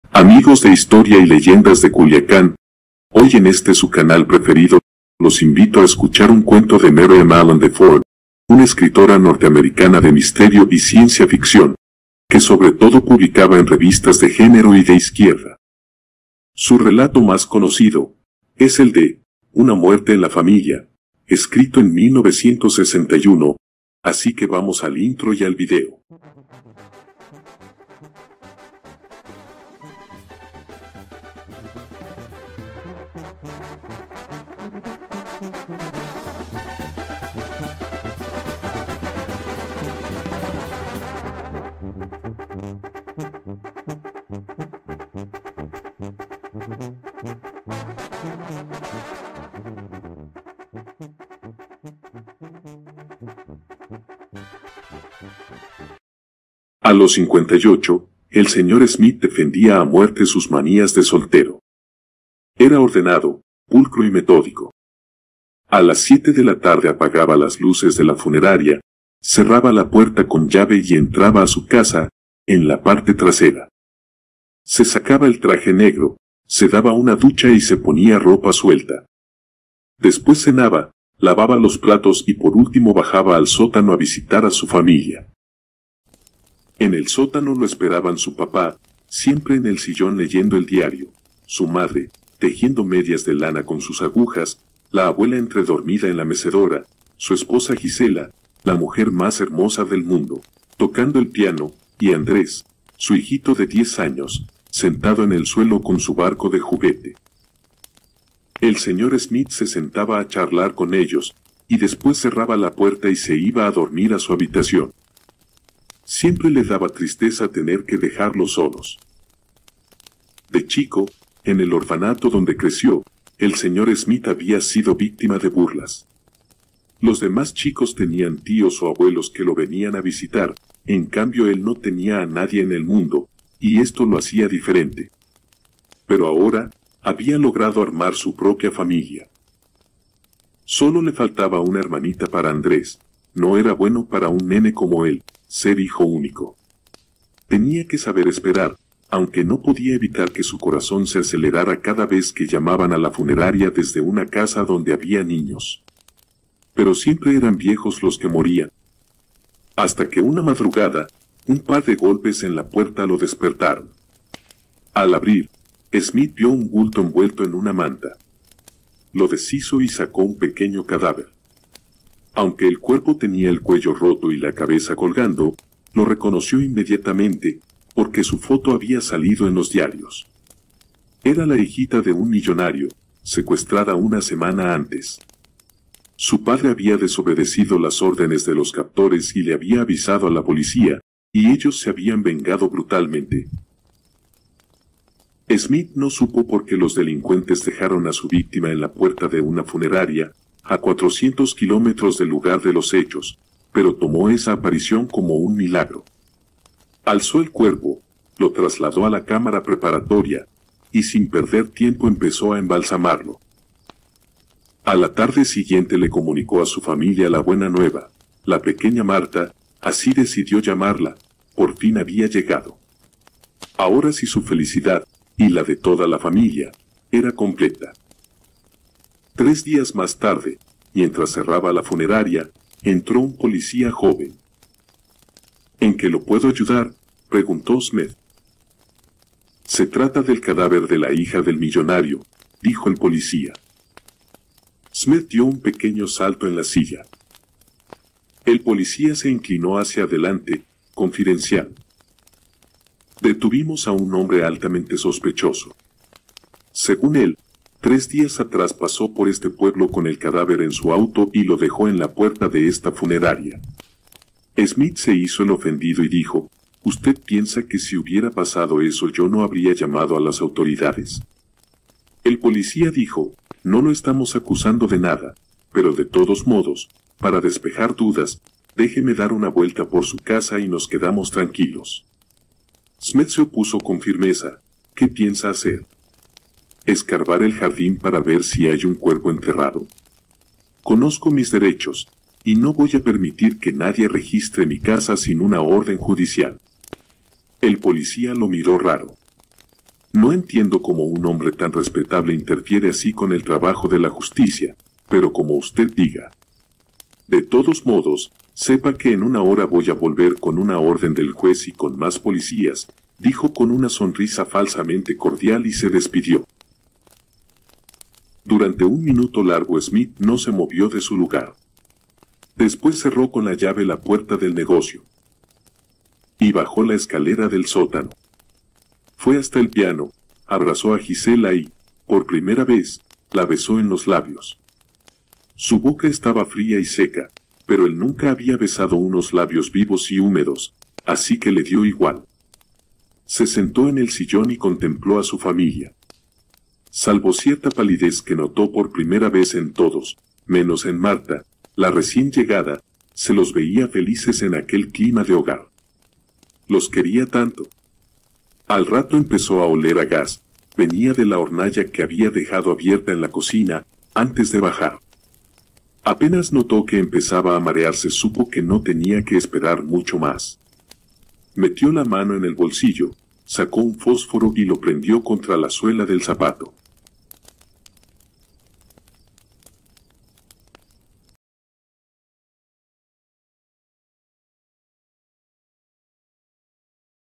Una muerte en la familia.
Amigos los invito a escuchar un cuento clásico de terror de Miriam Allen de Ford;, una escritora norteamericana de misterio y ciencia ficción.